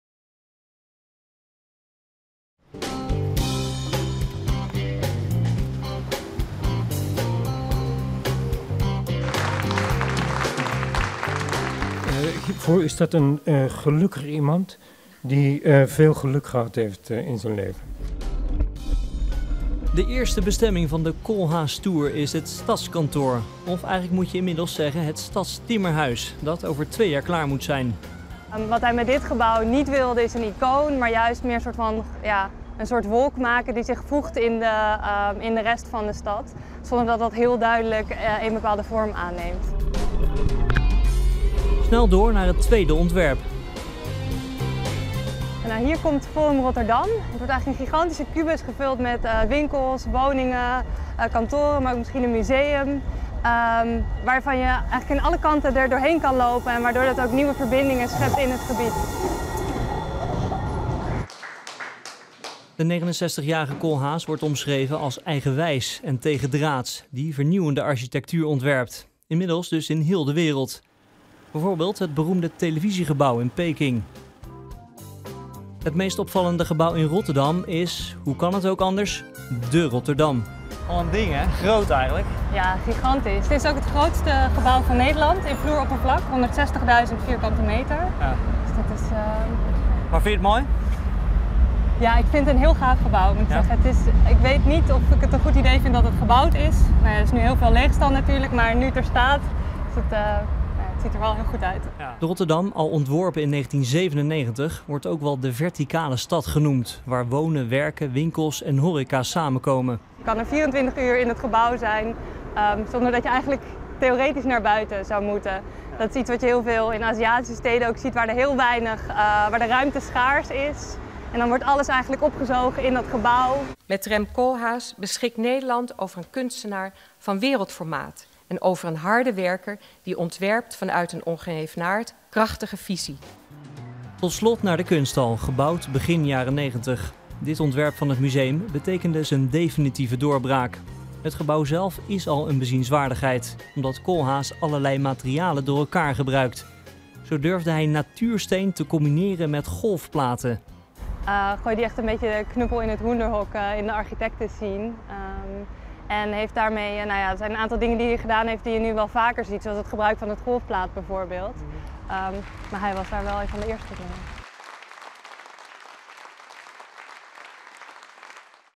Verslaggever Justus Cooiman maakte alvast een Koolhaas-fietstoer door Rotterdam
De Rotterdamse architect Rem Koolhaas krijgt waarschijnlijk een eigen toeristische excursie door Rotterdam en Den Haag. Het bedrijf Rotterdam Archiguides werkt aan een plan voor een rondje langs de verschillende gebouwen van de wereldberoemde architect.

   Lees meer: Rem Koolhaas-fietstour door Rotterdam